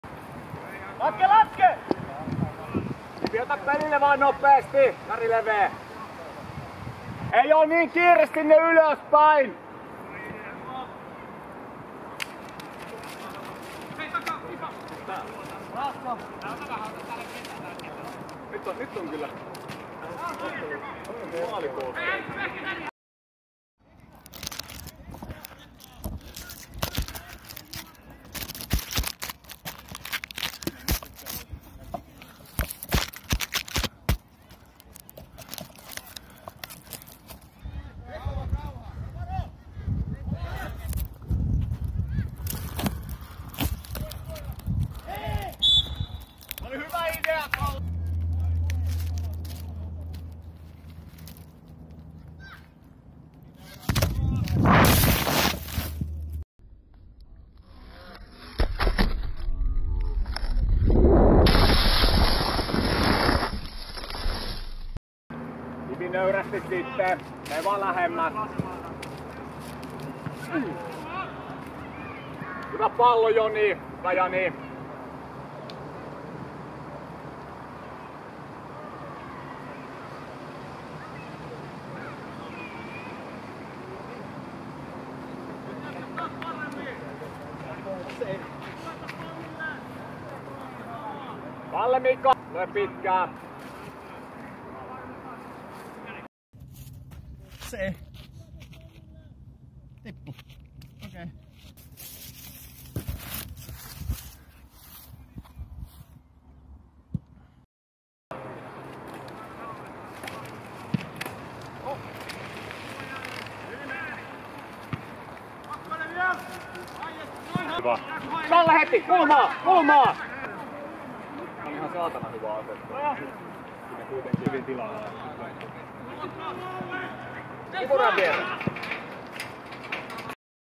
Behind the Scenes Mess
Onnistuneita vahinko-otoksia, huomioi myös ääniraita: Behind the Scenes Mess viime viikon POHU/Akatemia - SAPA -pelistä. Ennakkoon hyvissä ajoin valmistautuminenhan on aina tärkeää, eikös... ;p